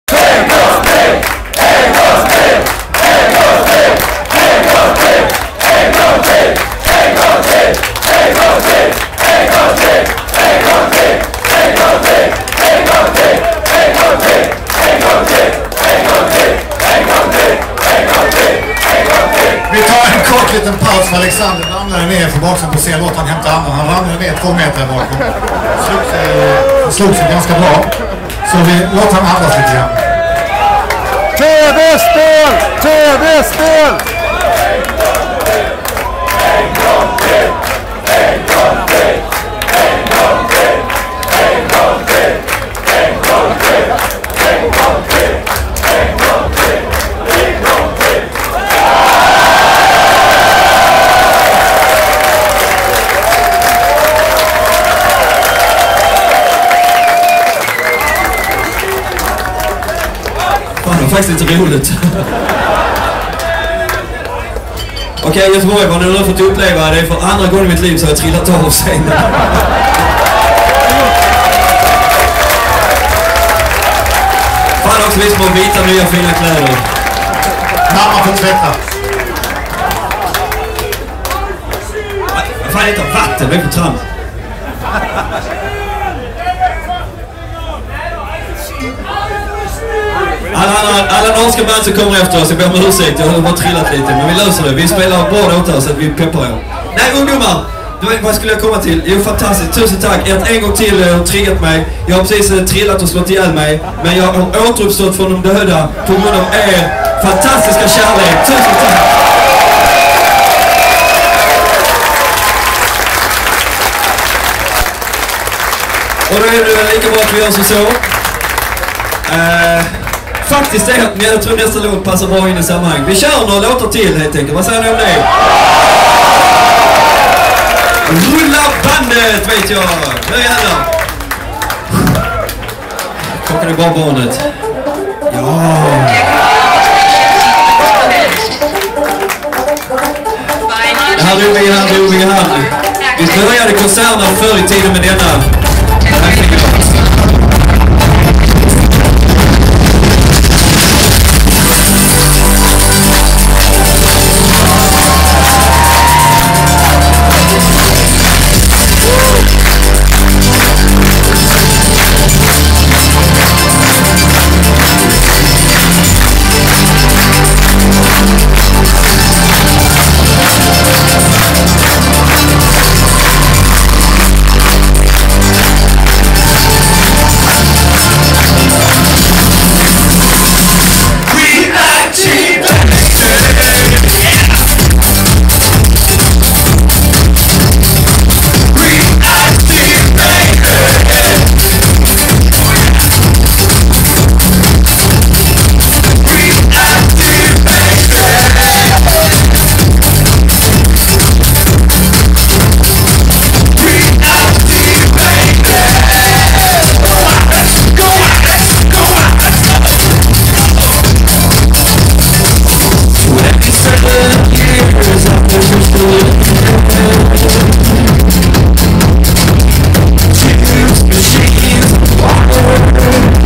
S.P.O.C.K. - Alexander ramlar av scenen i Göteborg
31:a augusti 2013
Usel gammal mobilkvalité